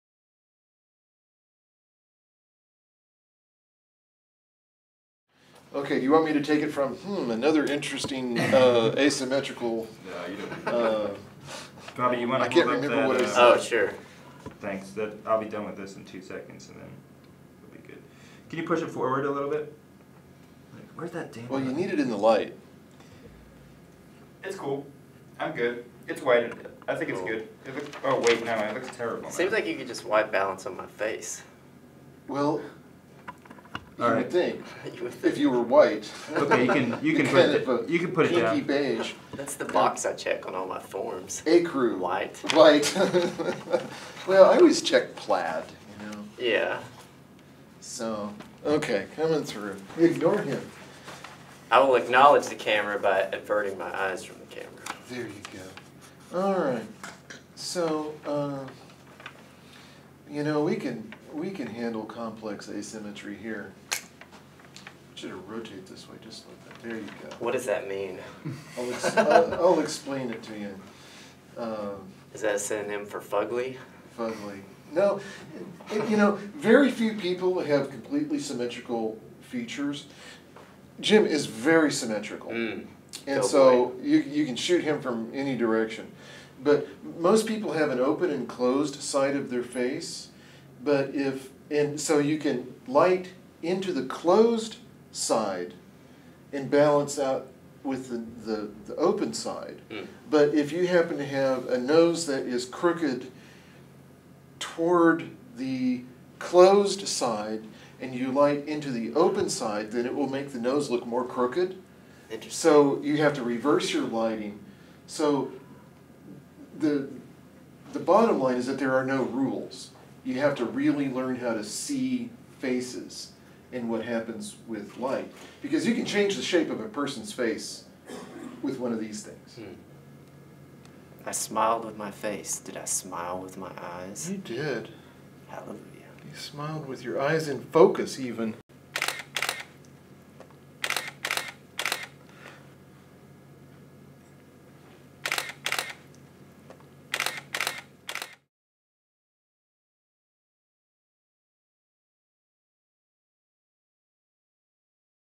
Complex Asymmetry
"Complex Asymmetry" is a short documentary that examines the art of portraiture.